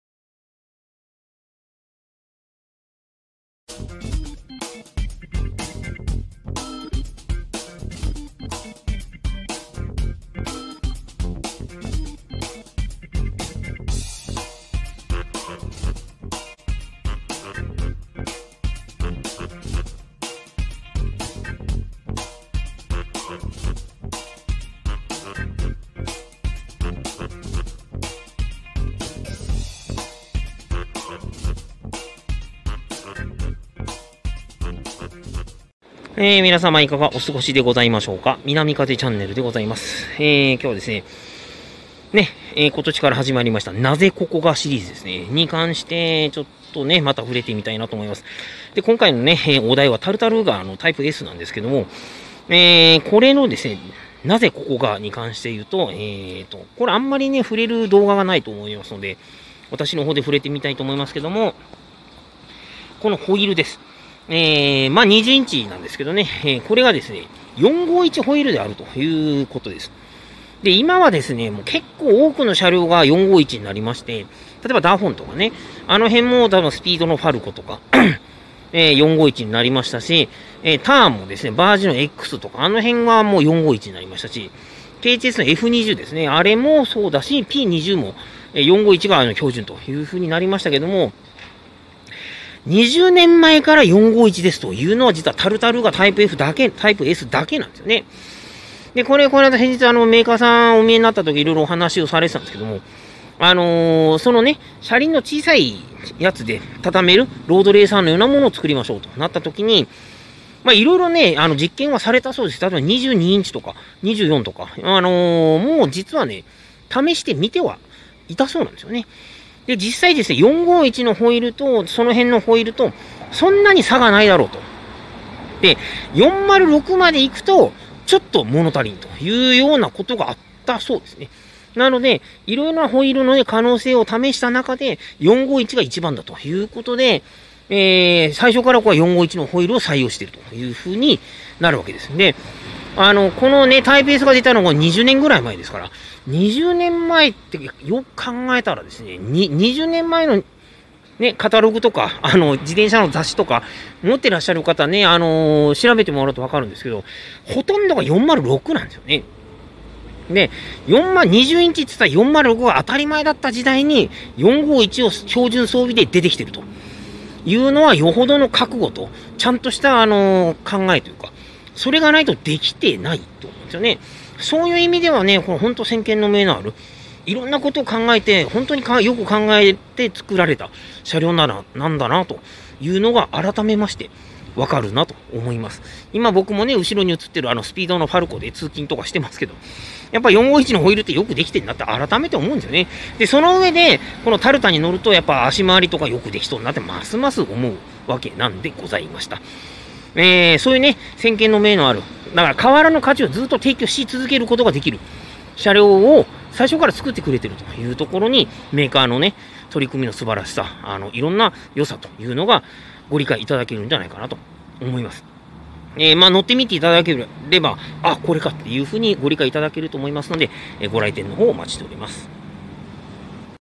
何故ここが、、４５１ホイル
言われてみれば、、なんですが、私の知る範囲で量産型の折畳車両で、４５１ホイルを最初に使ってきたのは本車両、、なんじゃなかろうかと思います。その慧眼を幾多の模倣者が追ってきている、、印象ですね。コメントは受け付けておりません。ご意見ご質問など店頭にてのみ承ります、ご来店をお待ちしております。